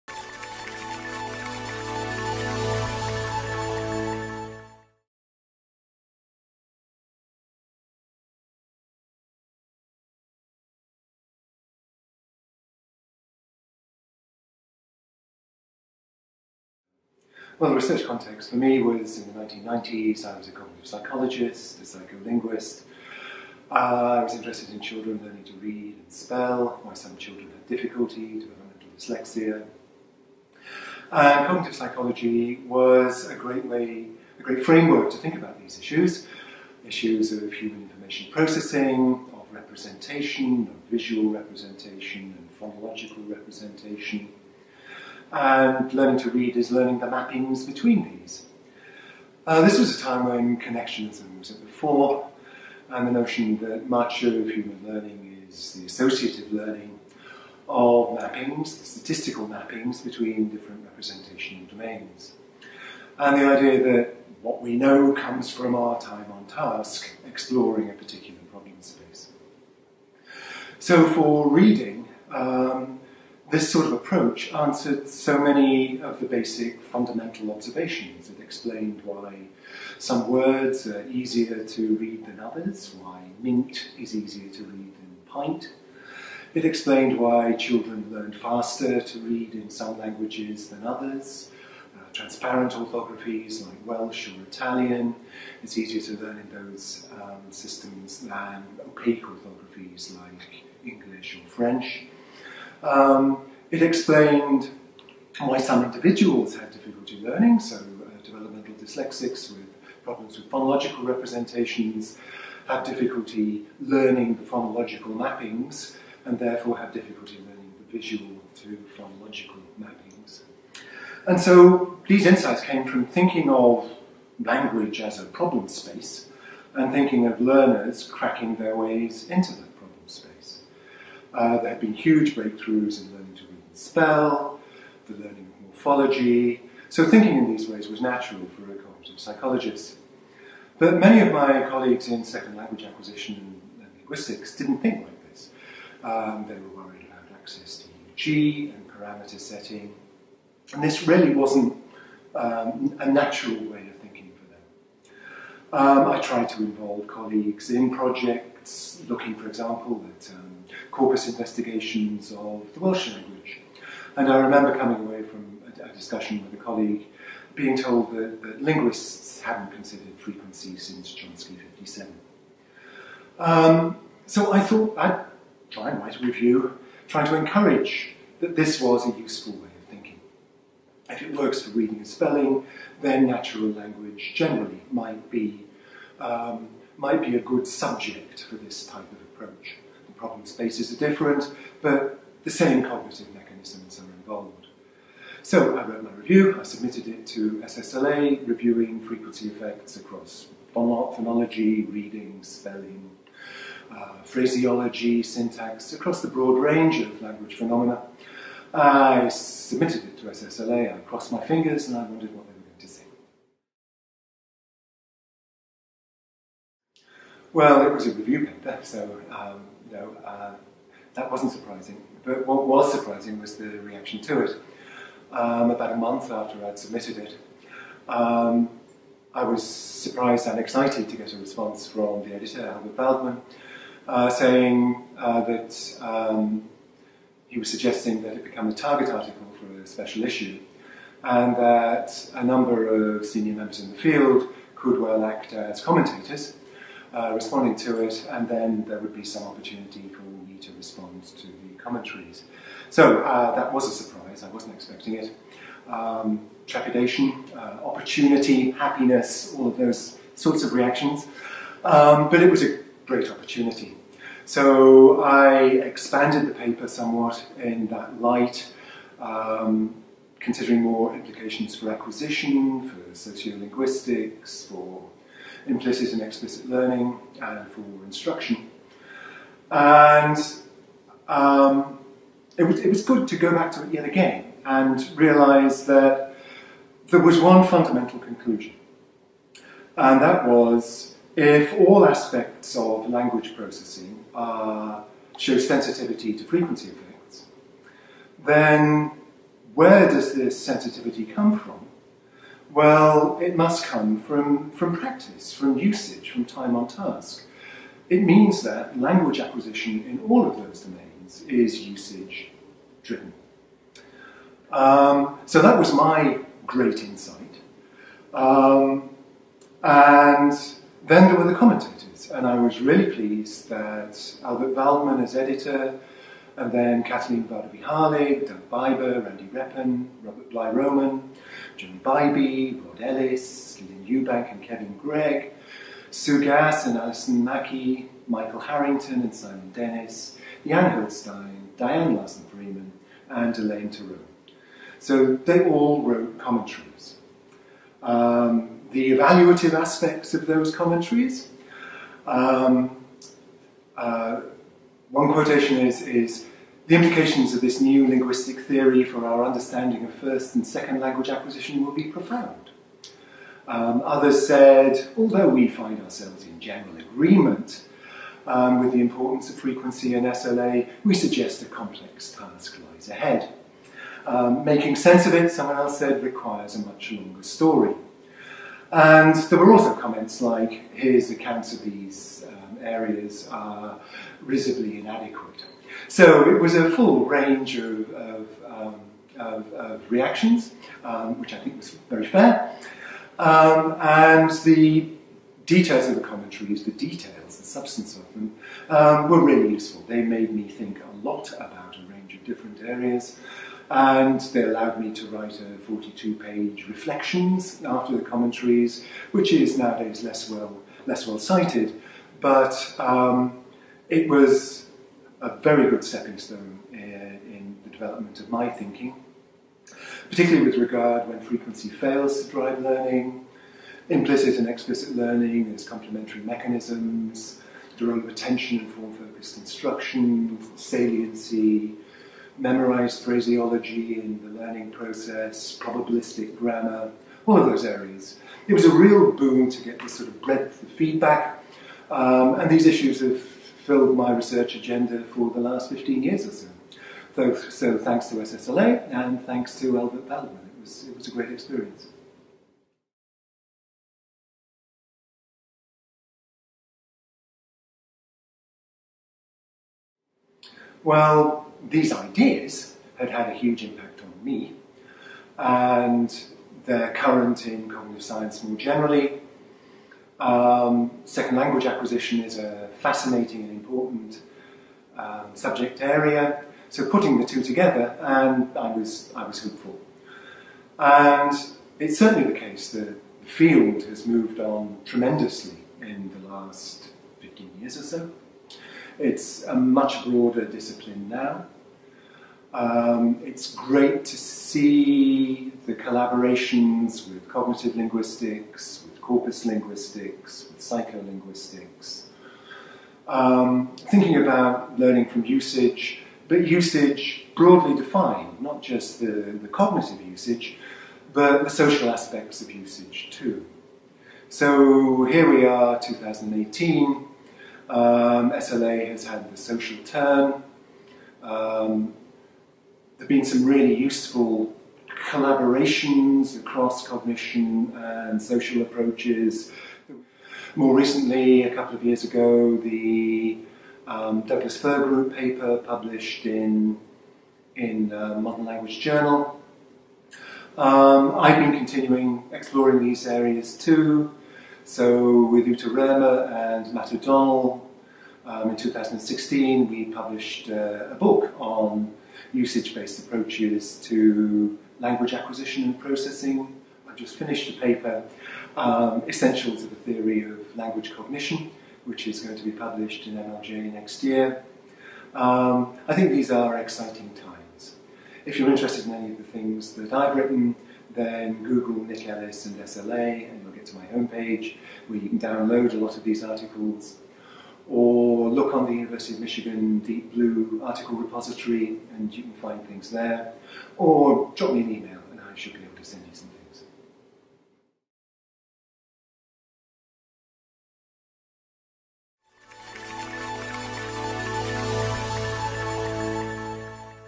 An interview with Nick Ellis for Studies in Second Language Acquisition
Nick discusses his featured article is'FREQUENCY EFFECTS IN LANGUAGE PROCESSING: A Review with Implications for Theories of Implicit and Explicit Language Acquisition'  Publication date: 2002 (Volume 24, Issue 2)